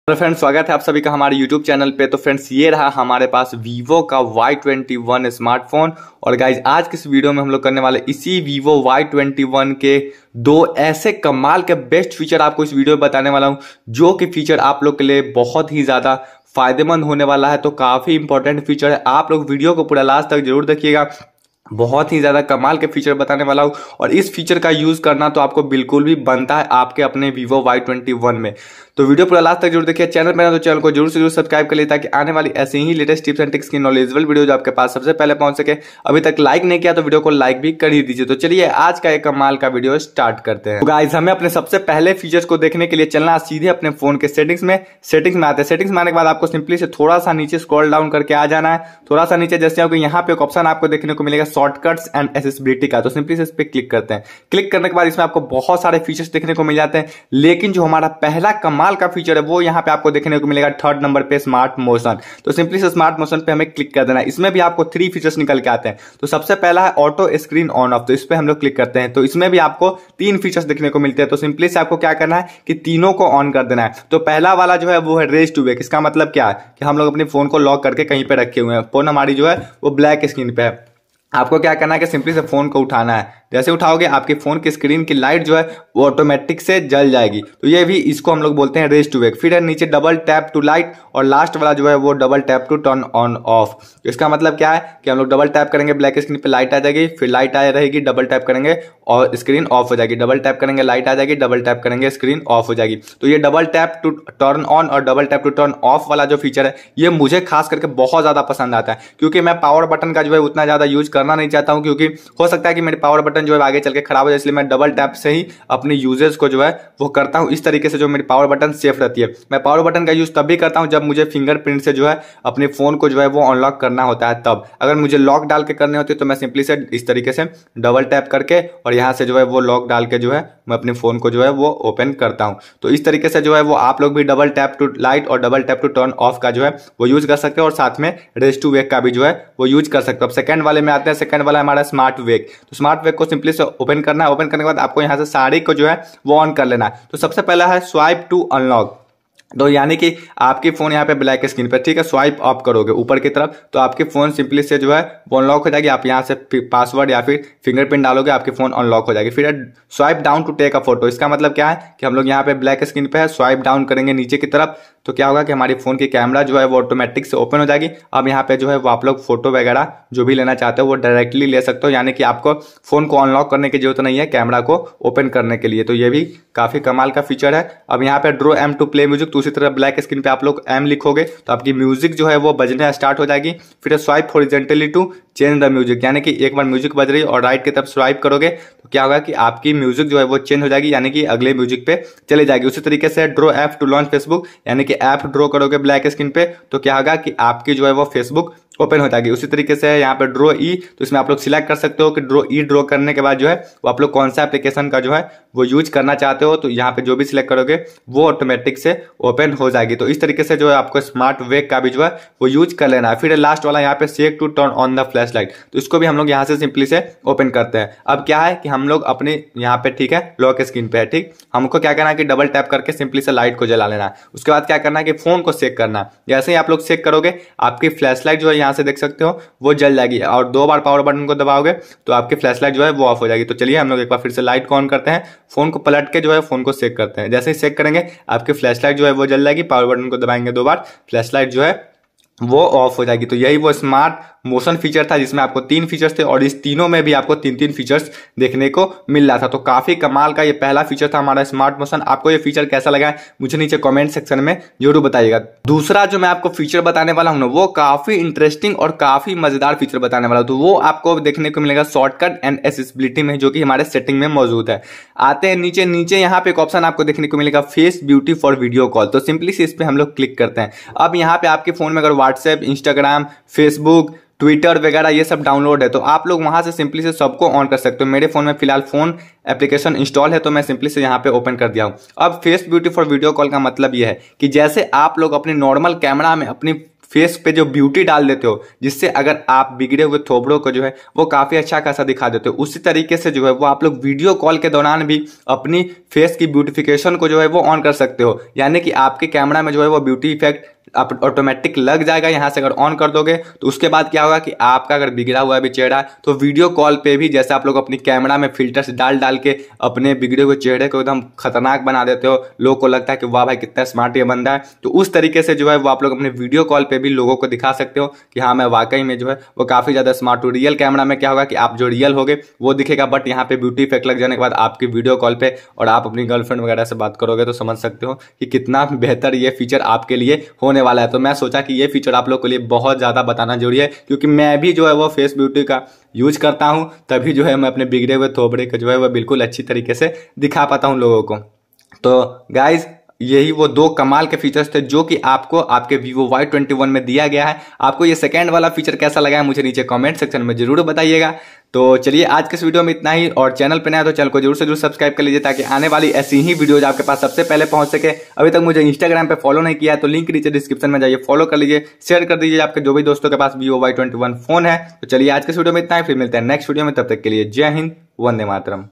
Vivo Y21 Top 2 Best features | tips and tricks
Friends aaj ki es video me hamlog dekhenge aur janenge vivo Y21 ke 2 kaafi kamal ke best features ke bare ki wo 2 features kon kon se hai aur unka uses kya hai to video kaafi kamal ka hone wala hai saath me kaafi knowledgeble v hone wala hai to pura last tak jarur dekhiyega